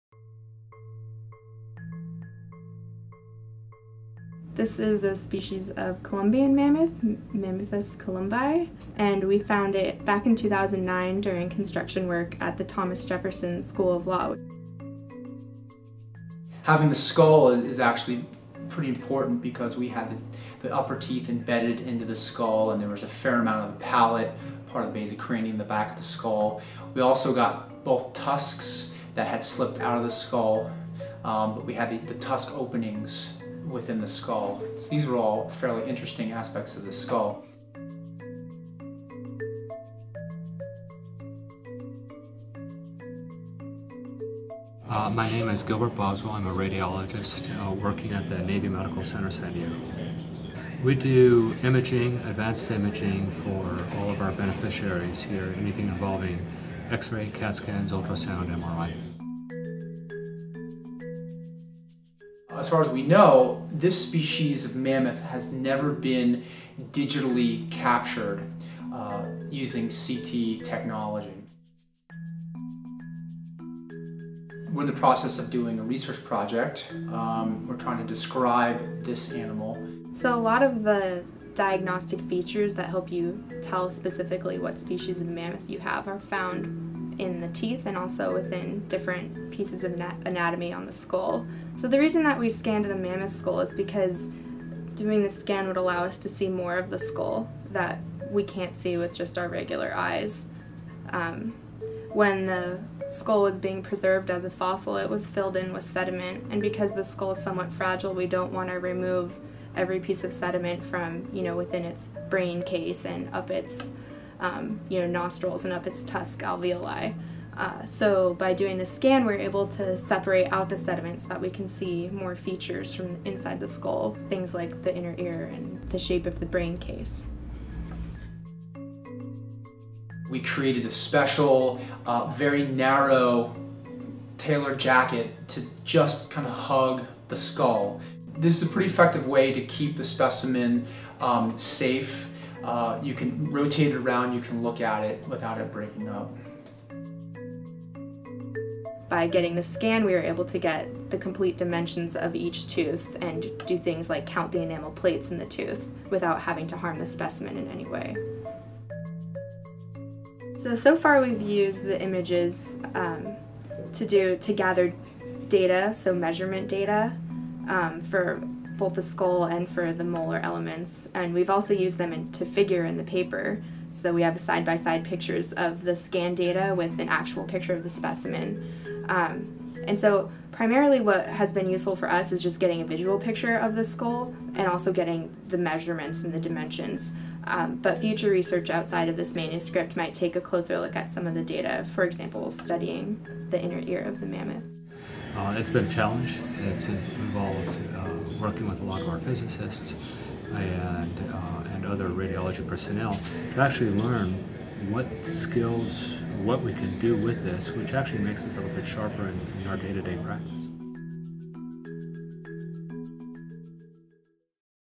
A Deep Look at a Mammoth’s Skull
Join paleontologists from theNAT as they collaborate with technicians from the Naval Medical Center San Diego to get a look inside the skull structure of a Columbian mammoth using high tech CT scanners.